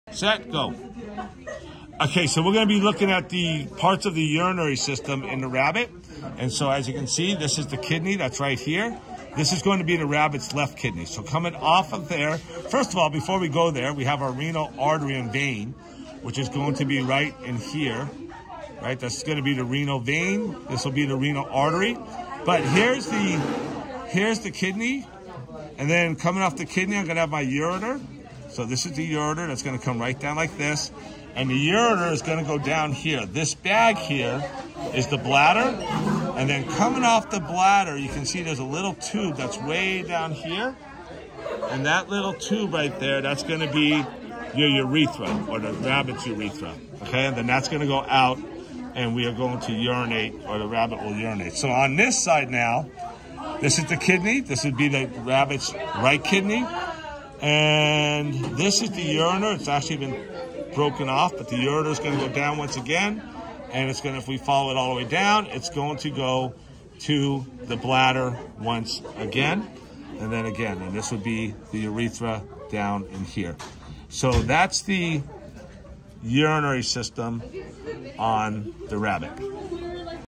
Rabbit dissection urinary system
This includes the kidney, ureter, urinary bladder and urethra